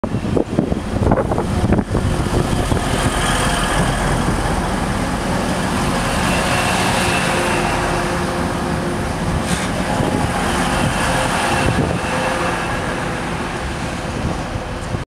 TPE Class 185 arriving at Stockport, WCML
Recorded at Stockport on the West Coast Main Line. This is an old clip, so I don't have as much info, therefore I had to look at the train itself for clues. Thank you all so much for watching and have a good day!